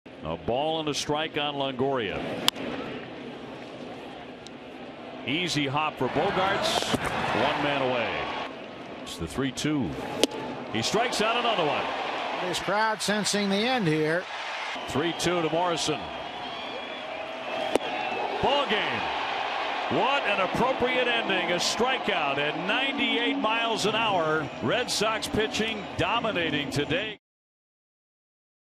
TB@BOS: Kimbrel secures win with flawless 9th
Craig Kimbrel strikes out a pair during his perfect 9th inning to earn the save in the Red Sox's 6-3 victory